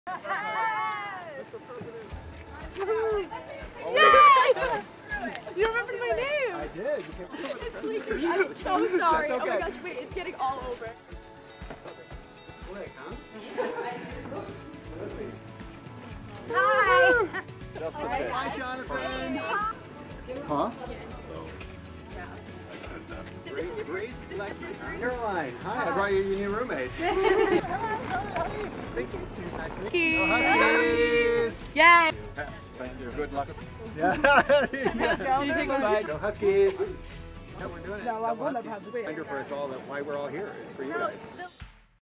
True Blue Move-In Crew | UConn
President Katsouleas spent the Friday of move-in weekend greeting first-year students and helping UConn23 move into their dorms.

FOLLOW UConn on social media!